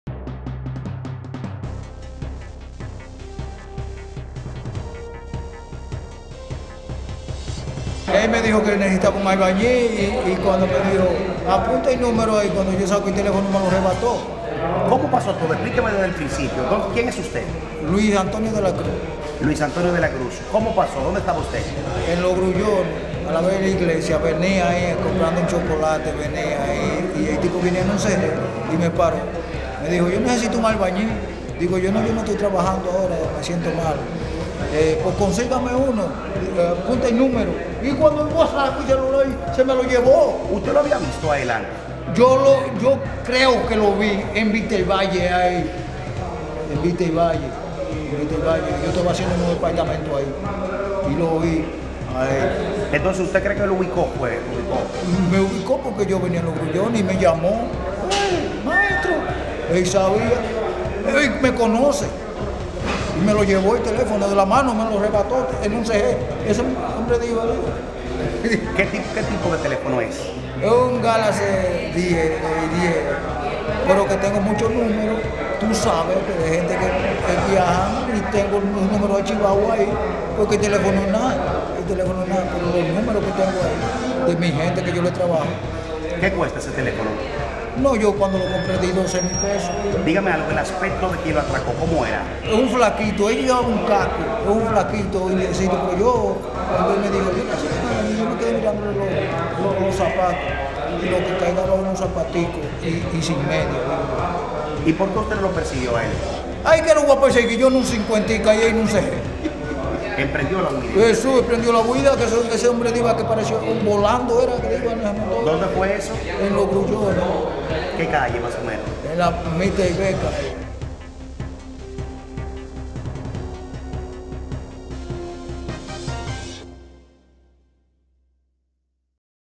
Albañil denuncia fue atracado por un hombre y lo despojó de su celular en SFM
San Francisco de Macorís.- Un maestro constructor narró cómo fue atracado por un motorista, que se le acercó cuando transitaba por la calle Mr. Beca del sector Ventura Grullón.

Luis Antonio de la Cruz dijo que el maleante lo detuvo para pedirle sus servicios de albañilería, y al decirle que no estaba trabajando, el ladrón le pidió el contacto de otro albañil y fue ahí cuando sacó el celular para darle el número y este se lo arrebató y escapó.

El denunciante dijo que el malhechor le llevó un teléfono marca Samsung modelo Galaxy 10, valorado en RD$ 12 mil, pero lo que más lamenta es que ha perdido todos los contactos de sus trabajadores y familiares, por lo que pidió ayuda a las autoridades para recuperar el equipo.